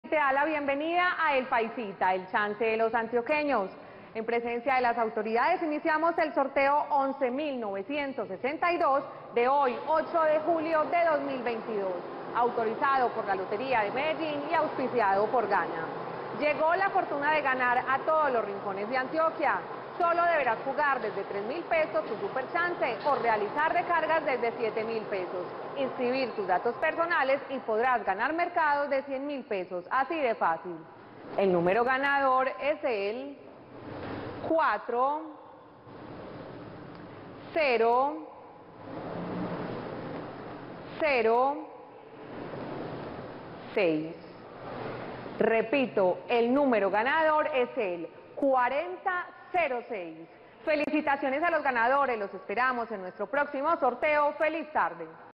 PAISITA DÍA Resultados del VIERNES 08 DE JULIO de 2022 ✅🥇🔥💰
Hola buenas tardes, te presentamos los resultados de las loterías y chances en Colombia, vamos con el Resultados del Chance Paisita día del VIERNES 08 DE JULIO de 2022.